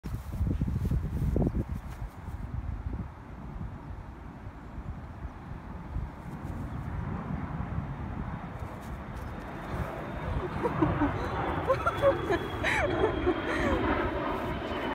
Bunny running super fast
My bunny, Bombón 💘, he is 7 months here. He is now 2 years old & still cute!